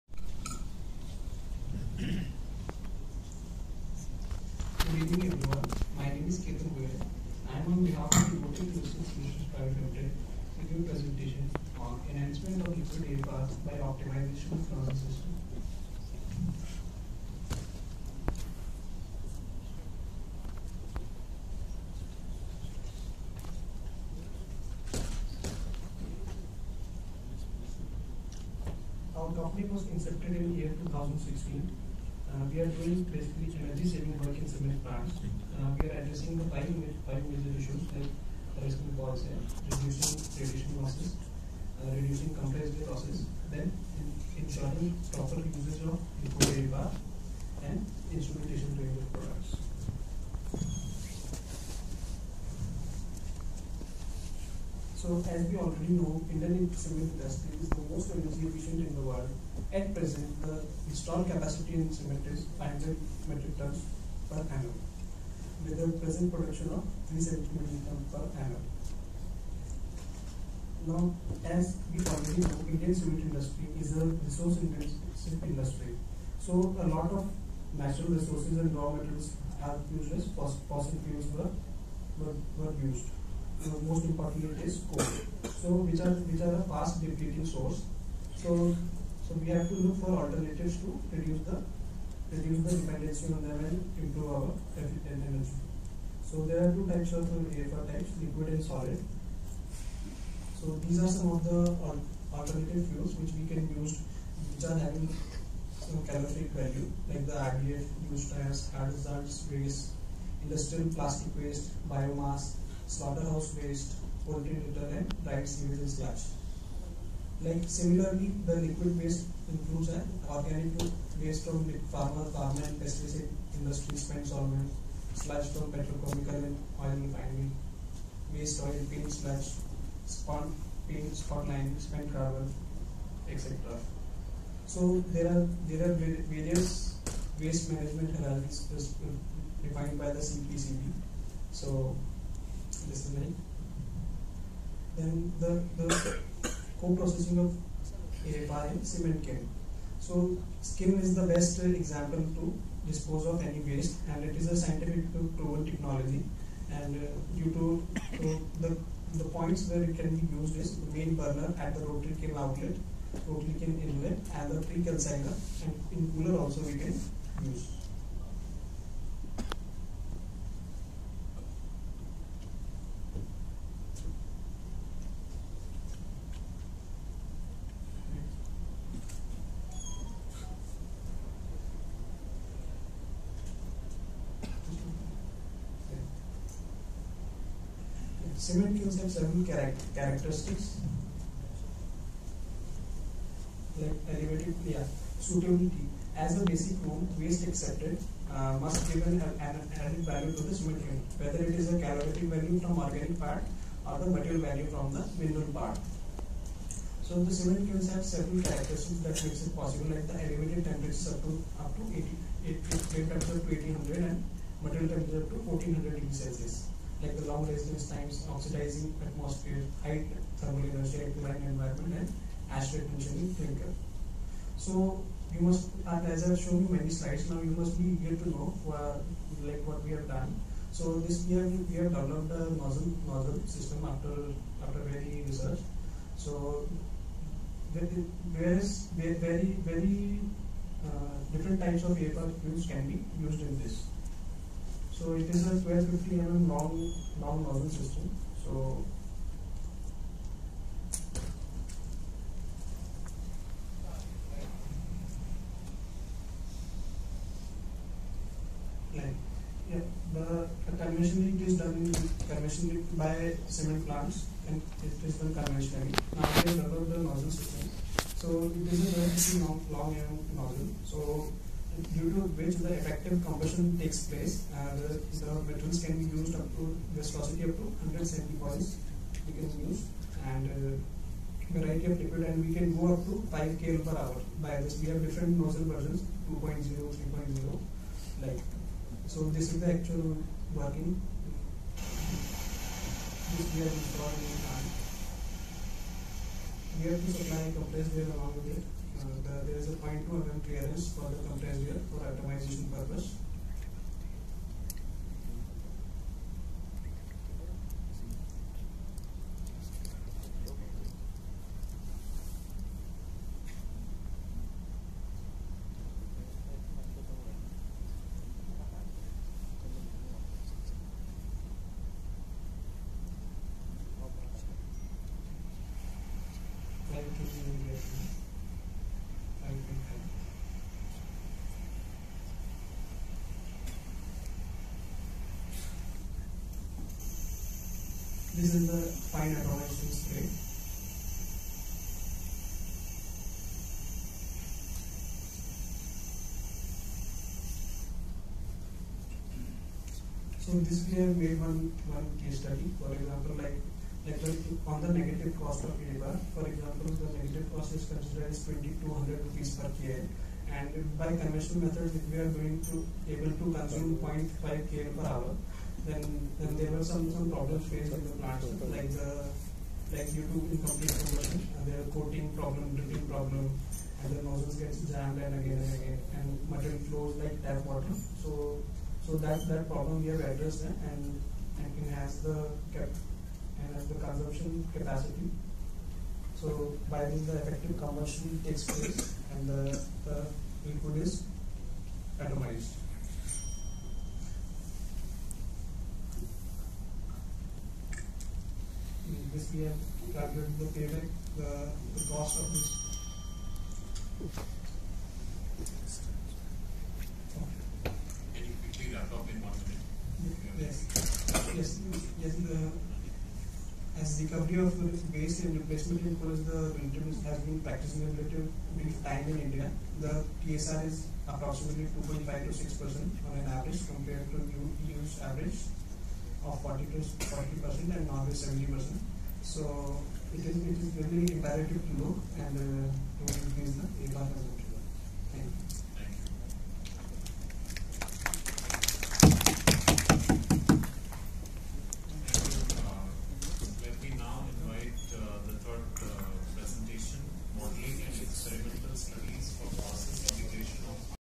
Presentation on "ENHANCEMENT OF LIQUID AFR BY OPTIMIZATION OF NOZZLE SYSTEM”.
17th NCB International Seminar on Cement, Concrete and Building Materials, 06-09 December 2022, New Delhi, India.

Low per capita consumption shows large growth potential in the sector in the future. The Indian cement industry consumes close to 77 million tons (Mt) of coal and 450-550 (Mt) of raw materials annually and is responsible for 7% of India's CO2 emissions.

Recovery of wastes and replacement of coal in the cement industry has been practiced in a relatively brief time in India, the Thermal Substitution Rate (TSR) is 2.5-6.0% on average; compared to the EU’s average of almost 40%, and Norway’s 70%. In a status report, published by CII in May 2015, the average TSR of 36 cement plants, corresponding to 109 Mt cement production capacity, was 3,55% (the highest single plant substitution is around 18% TSR). The Steel and Power industry has little experience in co-processing and is generally cautious to implement co-processing as they are afraid of possible negative impacts on their processes and their "product" quality. The Indian cement industry is gearing up with its infrastructure, capacity, and competence for enhancing thermal substitution rate (TSR), including the installation of pre-processing platforms and the adoption of newer technologies.

Implementation of effective waste management practices become imperative for the sustainable growth of the country and the cement industry, has proven the capability of providing safe, ecological sustaining, and environmentally sound solutions for the management of both Municipal and Industrial wastes - can supplement this requirement waste management in the country in an effective manner.

The cement industry is capable of co-processing wastes as alternative fuels and raw materials to reinforce its competitiveness and at the same time contribute to solutions to some of society’s waste problems in a way that valorizes the waste and is beneficial to the environment.